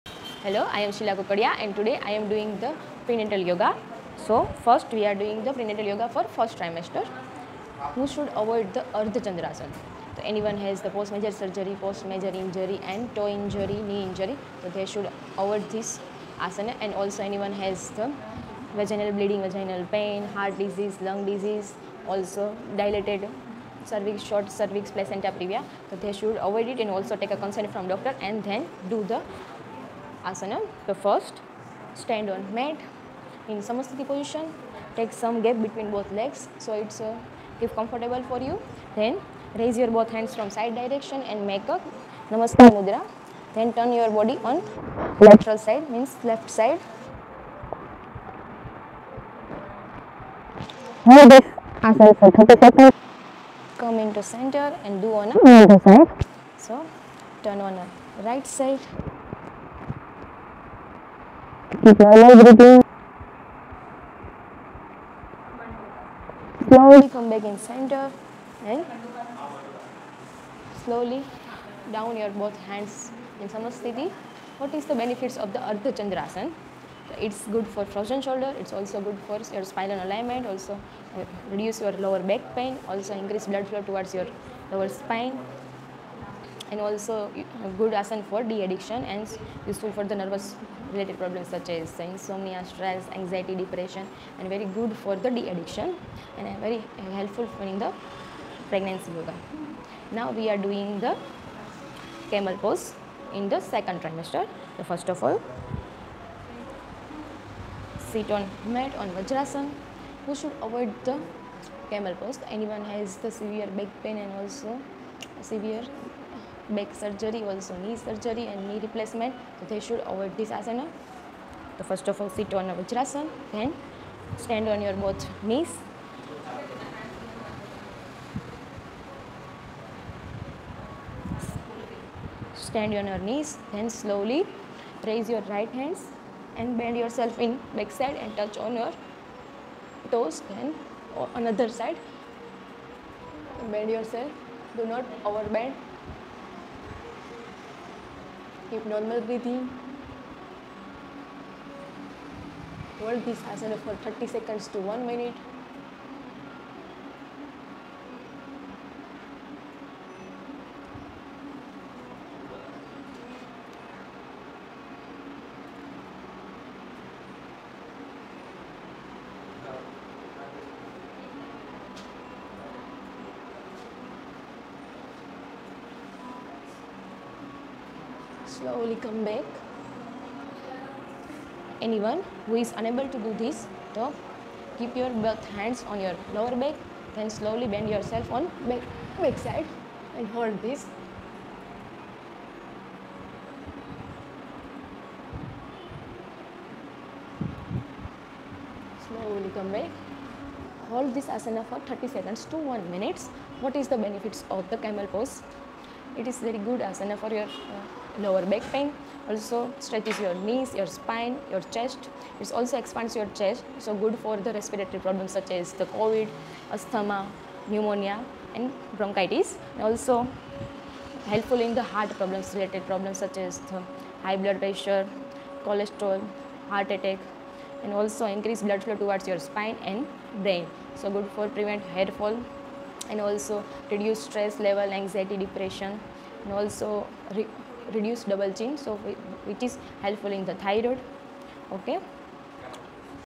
Dr Shila 1st & 2nd Trimester Prenatal Important Yoga Pose 4 every Mom
✓Get Reduced Inflammation, Vomiting & Swelling & Improved blood Circulation around baby.

✓Do not Ignore Yoga in 1st Trimester.. YogaWithSamarth When You & Baby need connection & Base for further journey

✓Practice Prenatal Yoga during 1st trimester too @exhaleyogaschool which most mom's ignore..

✓Get consent of doctor and Start Prenatal Yoga which has its own advantages for 1st trimester..

Join Trimester Specific Prenatal Yoga Classes & Courses at Exhale Yoga School

Yoga pose
Ardh Chandrasana & Ushtrasan (Camelpose)

learn amazing skilll & benefits during this tutorial